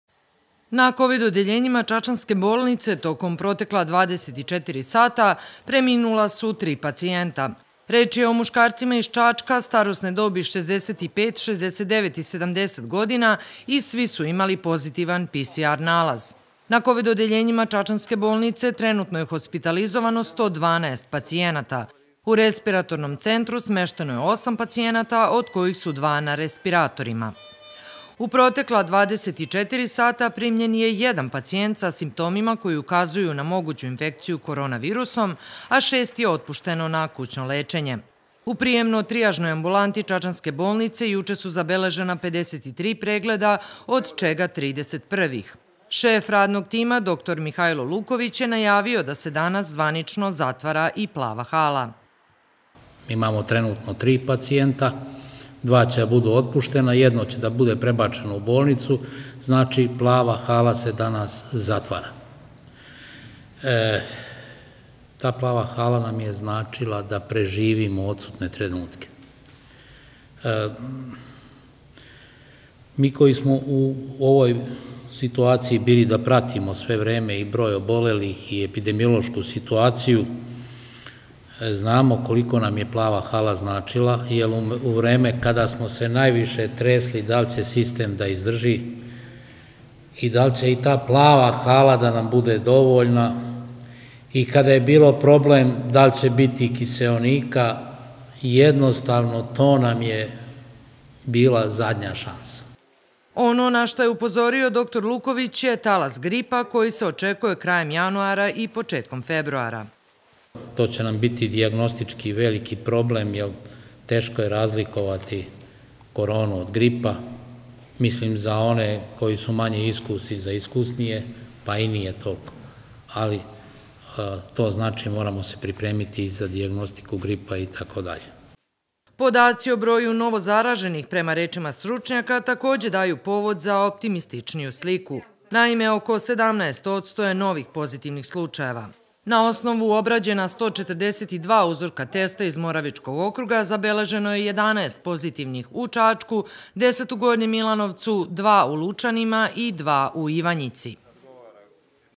TV Telemark - NA KOVID ODELJENJIMA U ČAČKU 112 PACIJENATA, DANAS SE ZATVARA PLAVA HALA 08 01 2021
Na kovid odeljenjima čačanske bolnice tokom protekla 24 sata preminula su tri pacijenta. Reč je o muškarcima iz Čačka starosne dobi 65, 69 i 70 godina i svi su imali pozitivan PCR nalaz. Na kovid odeljenjima čačanske bolnice trenutno je hospitalizovano 112 pacijenata.U Respiratornom centru smešteno je osam pacijenata, od kojih su dva na respiratorima.
U protekla 24 sata primljen je jedan pacijent sa simptomima koji ukazuju na moguću infekciju koronavirusom, a šest je otpušteno na kućno lečenje.
U prijemno-trijažnoj ambulanti čačanske bolnice juče su zabeležena 53 pregleda, od čega 30 prvih. Šeff radnog tima doktor Mihailo Luković je najavio da se danas zvanično zatvara i Plava hala.